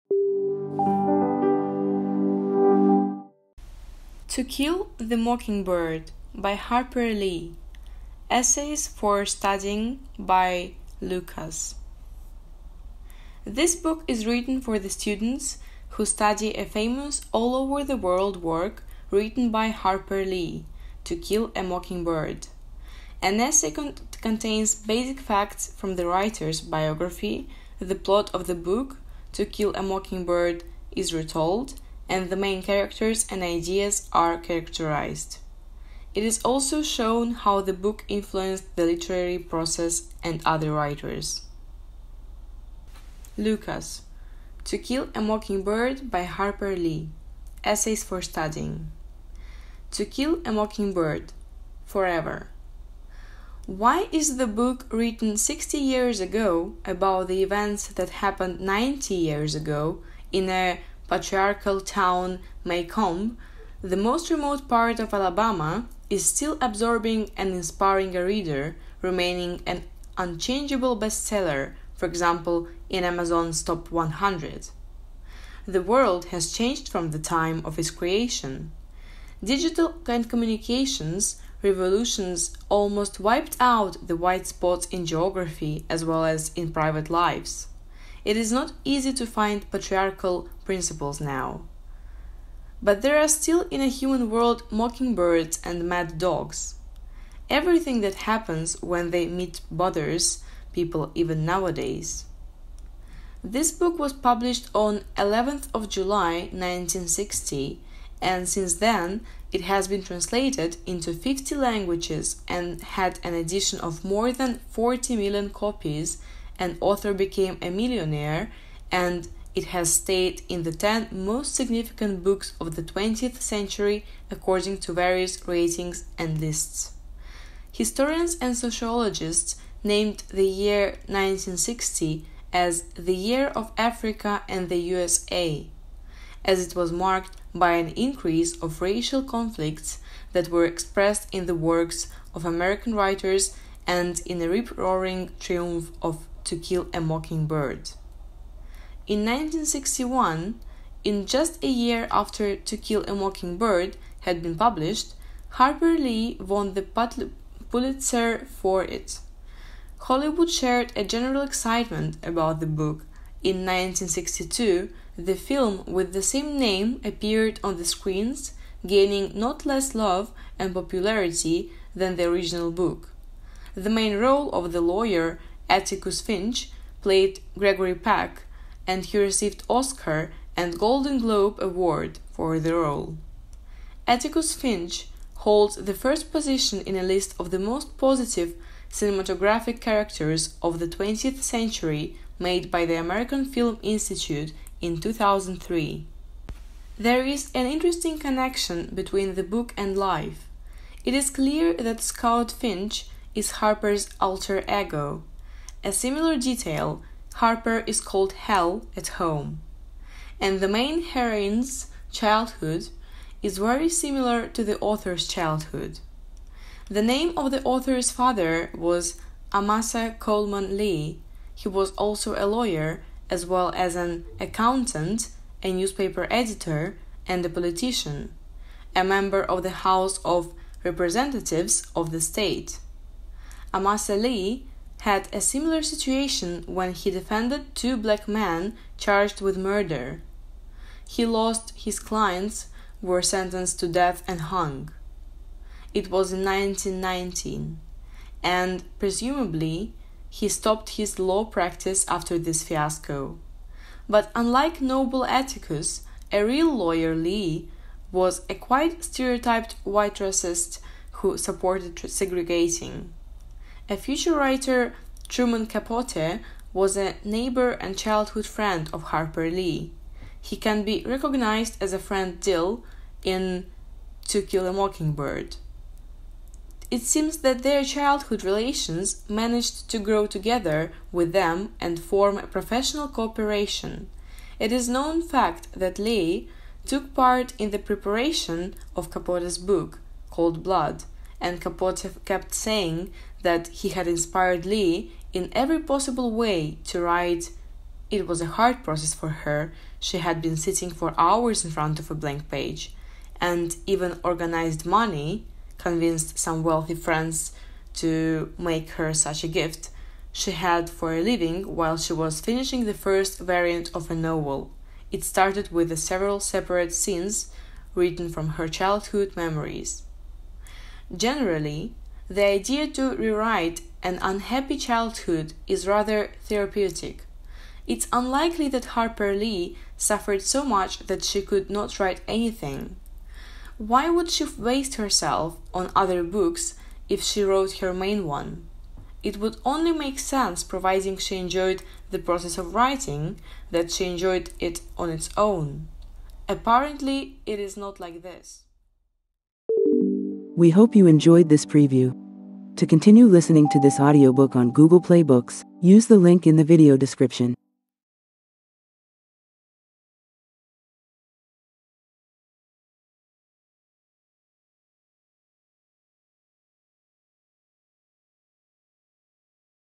The Essay for studying by Lukas: To Kill a… by Lukas · Audiobook preview
The Essay for studying by Lukas: To Kill a Mockingbird by Harper Lee
Authored by Lukas
Narrated by Mary Kay

BOOK DESCRIPTION

This book is written for the students who study a famous all over the world work, written by Harper Lee, To Kill a Mockingbird An essay contains basic facts from the writer’s biography, the plot of the book To Kill a Mockingbird is retold, and the main characters and ideas are characterized. It is also shown how the book influenced the literary process and other writers.

AUDIOBOOK DETAILS

Language: English
Publisher: Strelbytskyy Multimedia Publishing
Published on: December 20, 2020
Duration: 0 hr, 21 min
Genres: Fiction / Classics, Fiction / Literary, Fiction / Short Stories (single author), Literary Collections / General